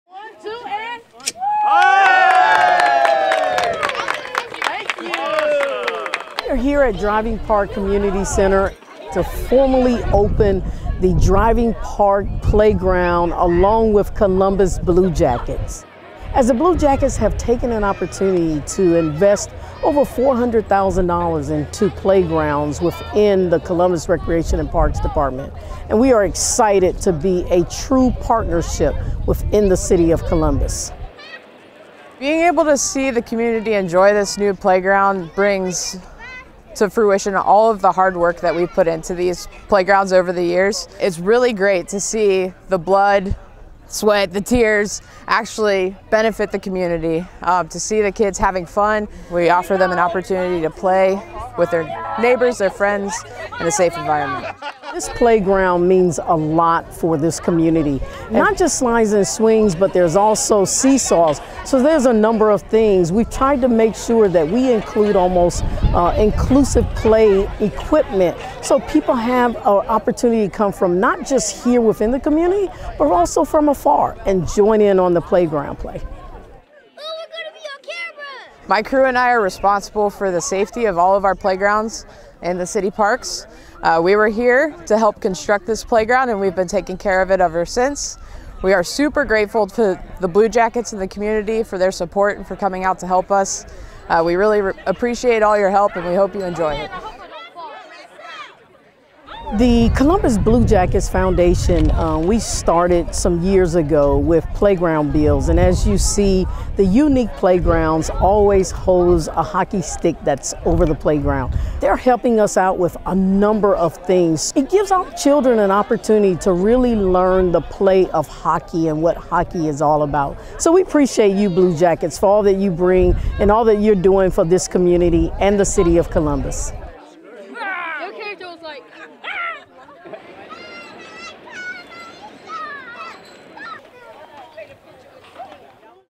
Dedicating the Columbus Blue Jackets Playground at Driving Park
The Columbus Blue Jackets Foundation dedicated a brand-new playground at Driving Park on April 11, 2023. This new playground is a great addition to the community!